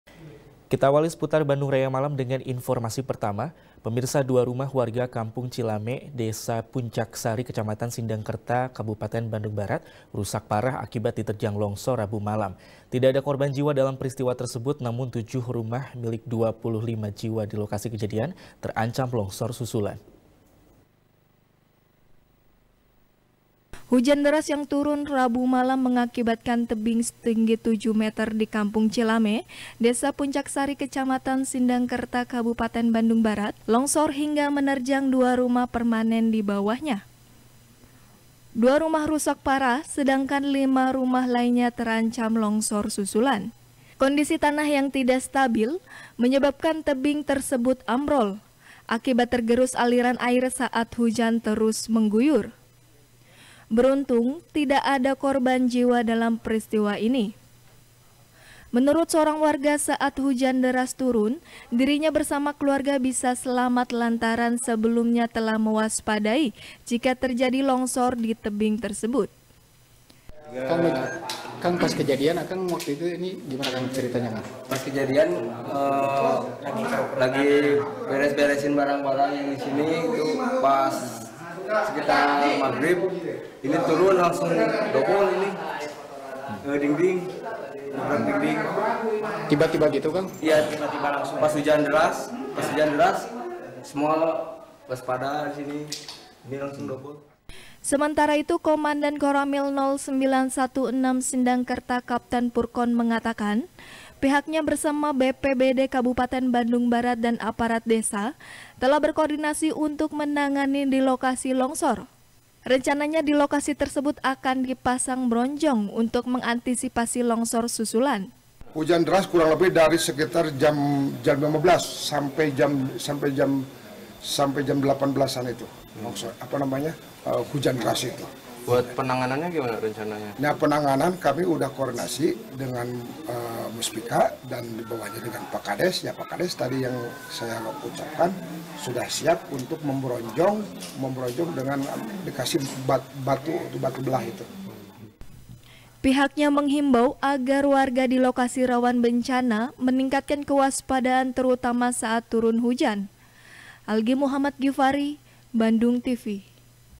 2 Rumah Rusak Parah Diterjang Longsor | SBR | BANDUNGTV
SBR | BANDUNGTV | IRFAN RAMANDA | Dua rumah warga kampung cilame, desa puncaksari, kecamatan sindangkerta, kabupaten bandung barat, rusak parah akibat diterjang longsor, rabu malam. tidak ada korban jiwa dalam peristiwa tersebut. namun, tujuh rumah milik dua puluh lima jiwa di lokasi kejadian, terancam longsor susulan.